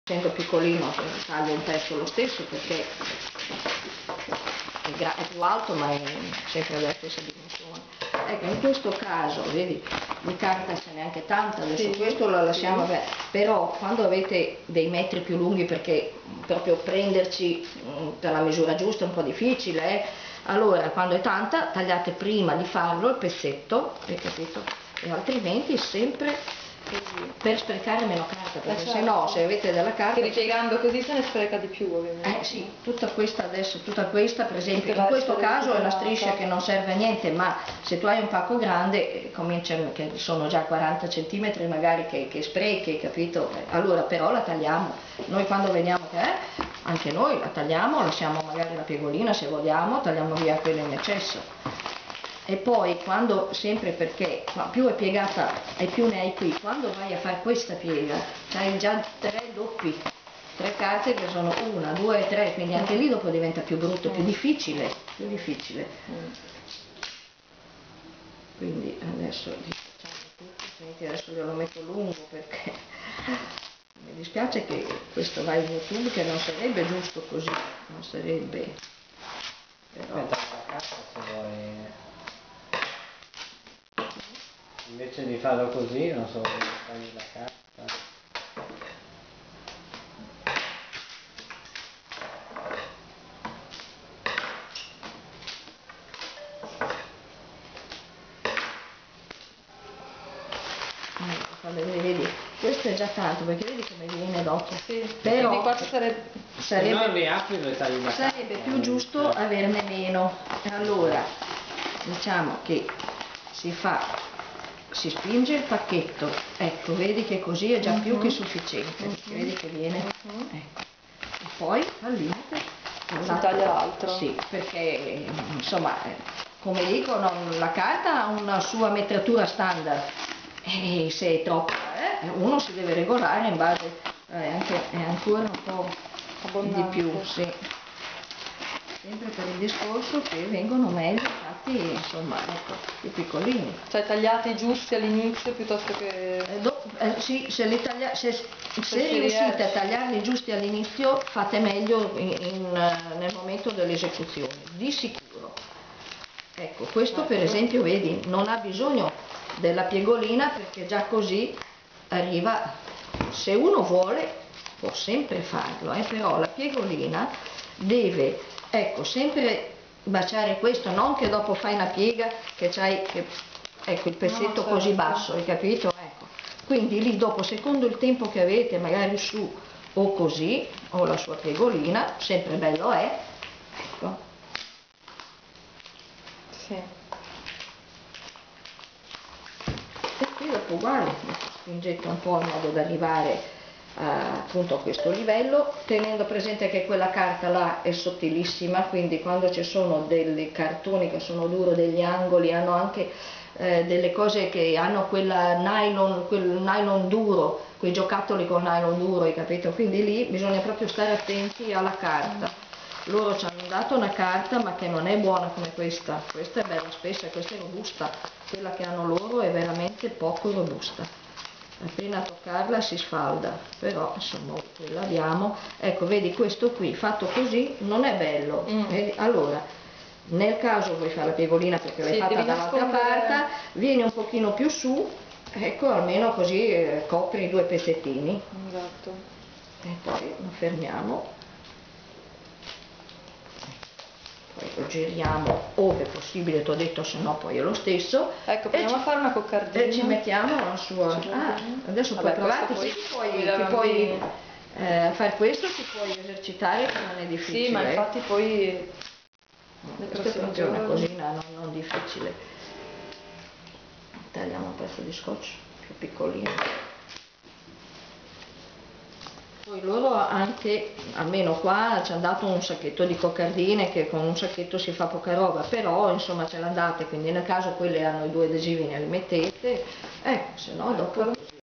IBO Italia presenta_I pacchi di Silvana_pacco medio
Silvana, volontaria di IBO Italia, insegna a fare i pacchi di Natale. Guardalo, impara e partecipa all'iniziativa"Fai un pacco...per solidarietà" promossa da IBO Italia